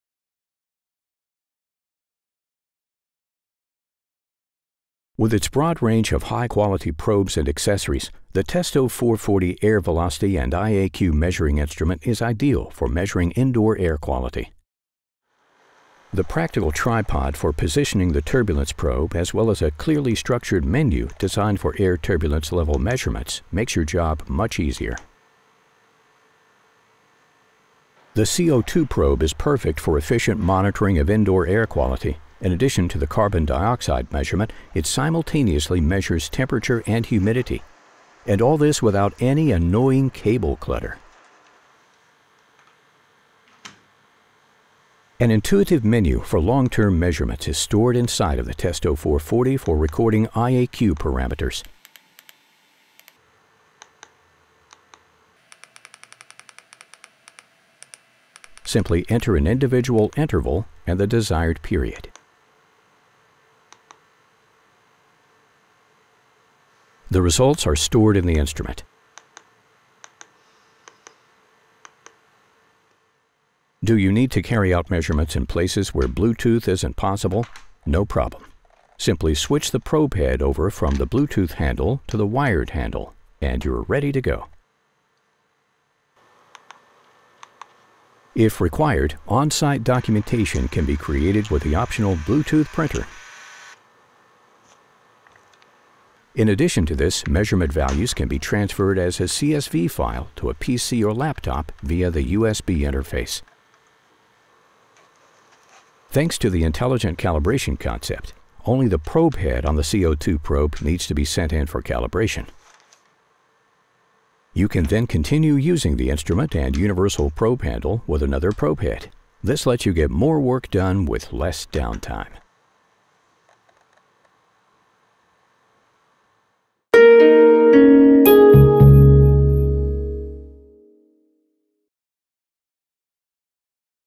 Measuring Indoor Air Quality and Comfort Level - testo 440
Measuring indoor air quality and comfort level using the testo 440 IAQ meter and data logger.